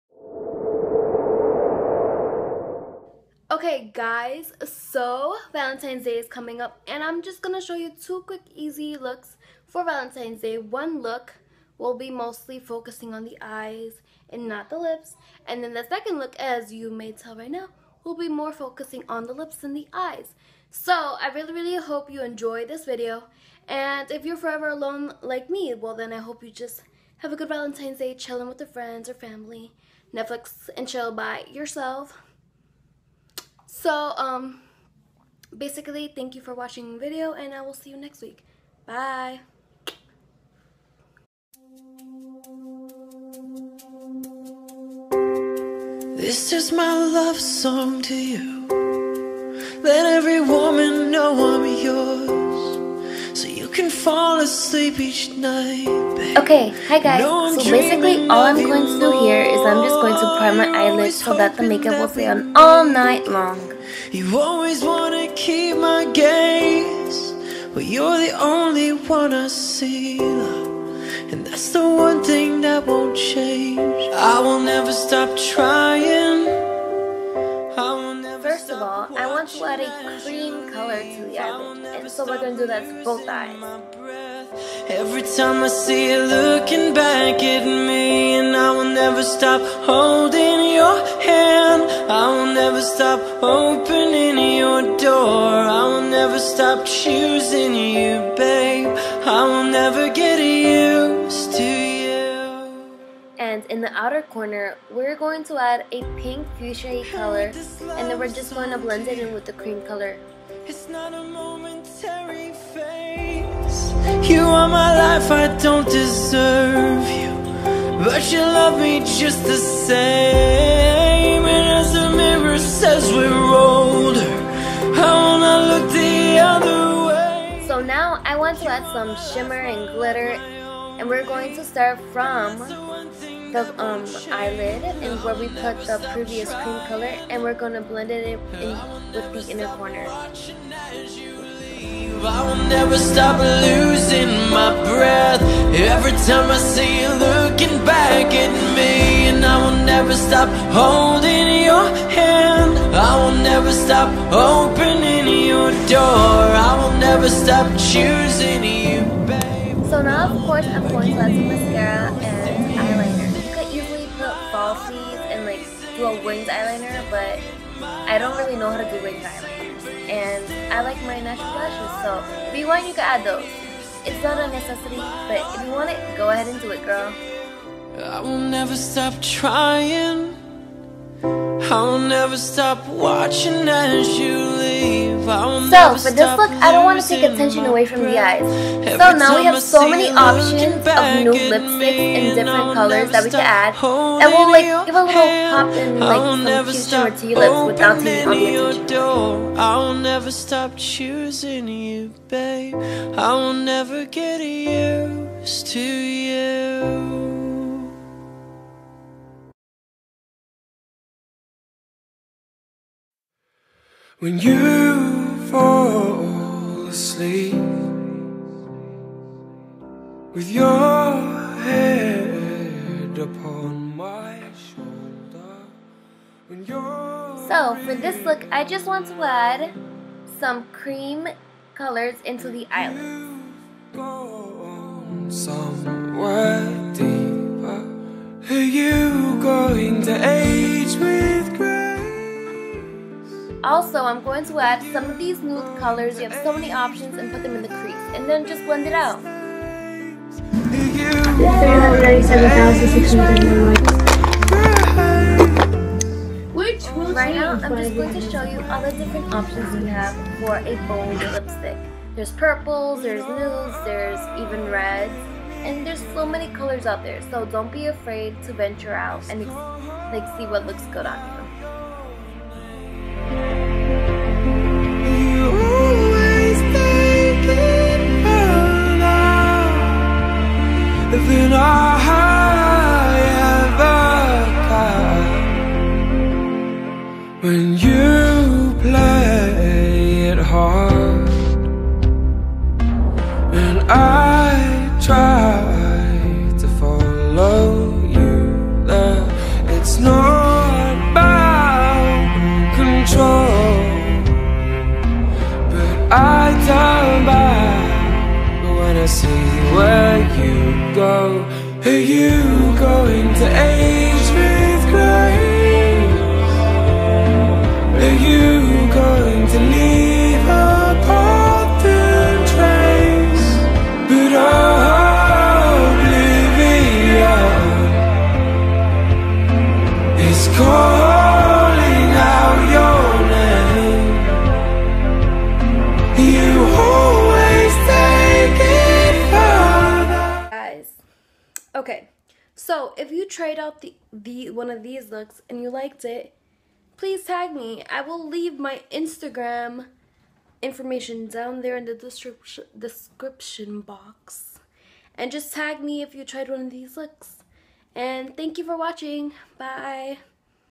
2 Valentines Day Looks | The Lost Girls
Hey lovely Losties!
Valentines day is right around the corner, so I put together two simple makeup looks that are a nice for chilled day out.
 Don't forget to tune in everyday for a new video.